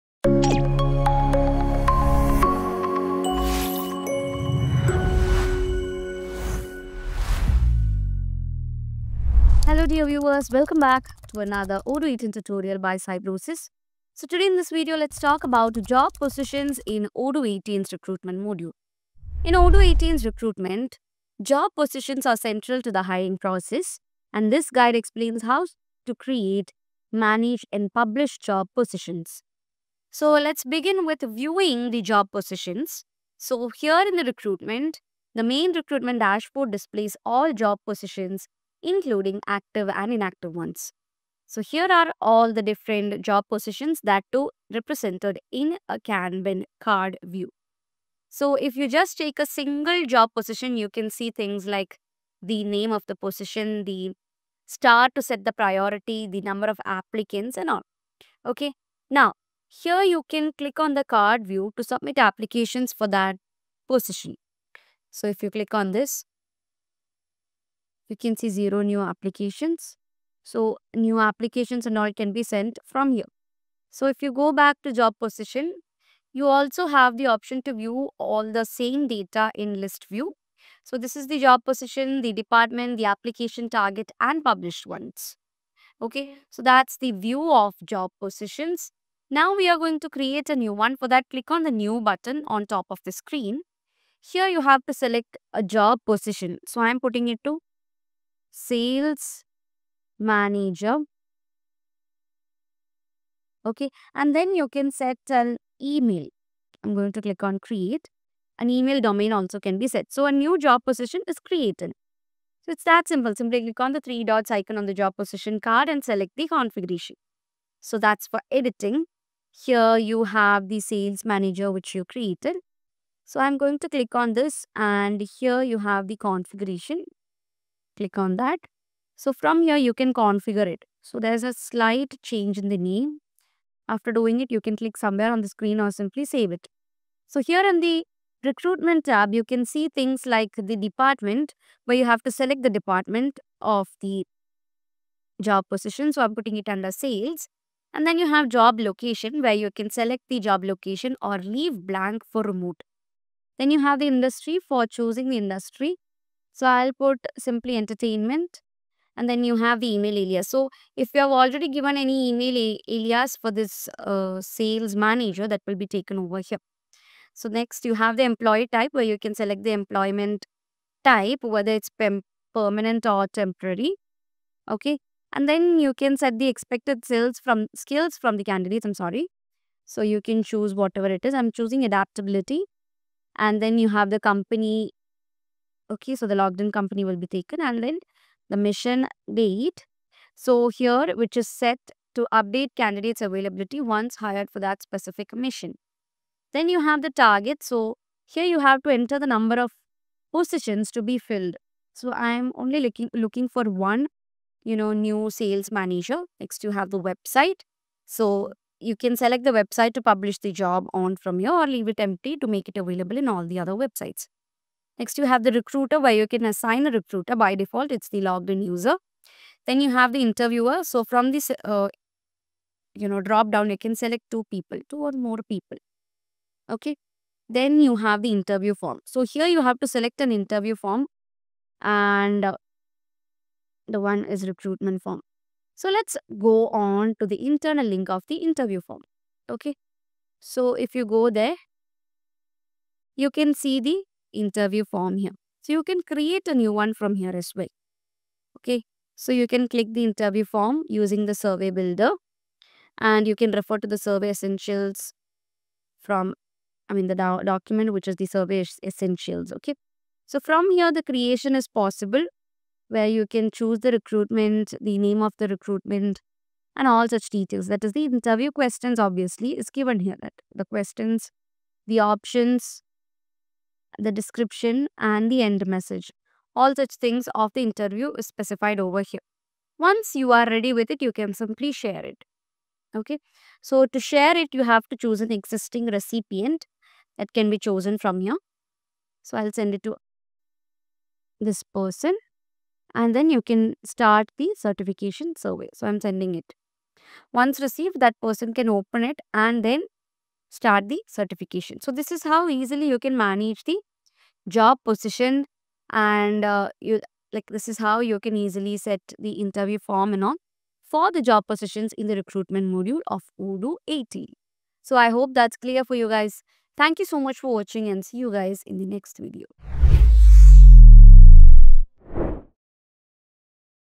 How to Manage & Publish Job Positions in Odoo 18 Recruitment | Odoo 18 Recruitment Tutorials
Job Positions in Odoo 18 Recruitment

Job Positions are the core element of the recruitment process in Odoo 18. They represent the specific roles and openings within your company.

Creation: Easily create new job positions with key details like job title, location, department, and employment type.
Management: Manage all aspects of a job position, including its status (active/inactive), publishing settings, and assigned recruiters.
Applicant Tracking: Track all applications for a specific job position, including applicant details, interview schedules, and hiring decisions.
Customization: To attract the right candidates, customize job position details, including job descriptions, required skills, and salary ranges.
By effectively managing job positions within Odoo 18 Recruitment, you can streamline your hiring process, attract top talent, and make informed hiring decisions.